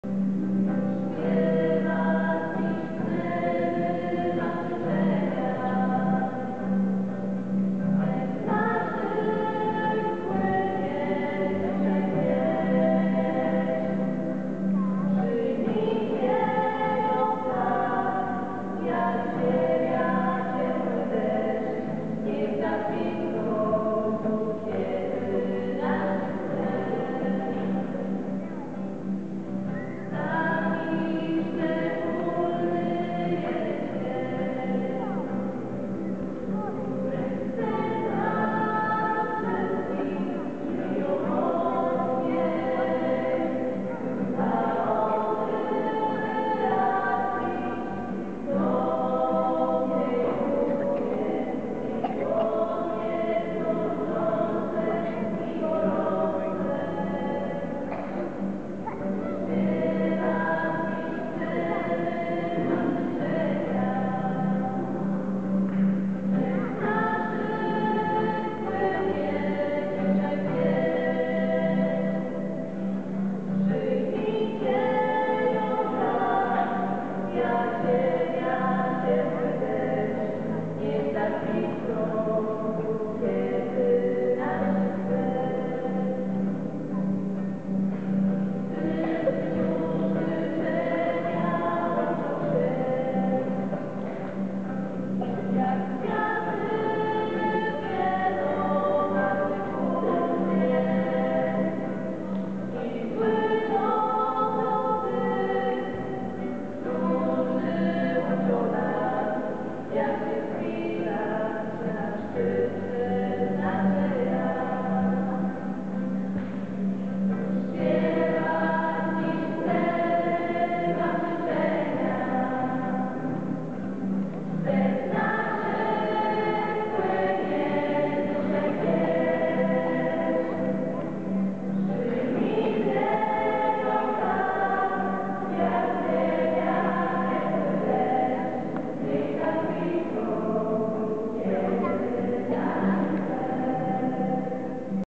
RUTKA-TARTAK - DZIEŃ NAUCZYCIELA 2010
Aneta, Magda & Tomek performance recorded by Krzysztof Masłowski